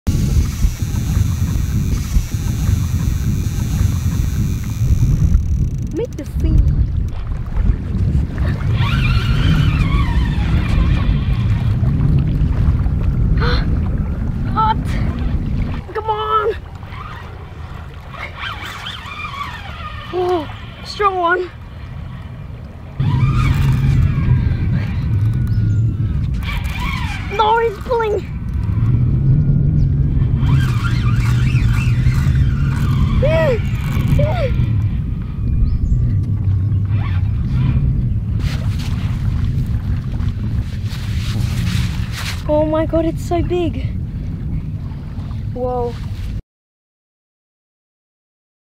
Monster Fish!! Land based Fishing in Queensland, Australia
Fishing shorts in Queensland, Australia. Land based fishing and caught a big blue salmon. How to get a monster fish by 11 years old. We normally fishing from beach land based and catch Bream, Whiting, Trevally, Queen fish, Cod etc.. today is lucky day catching this big Blue Salmon!

Full version of this video▼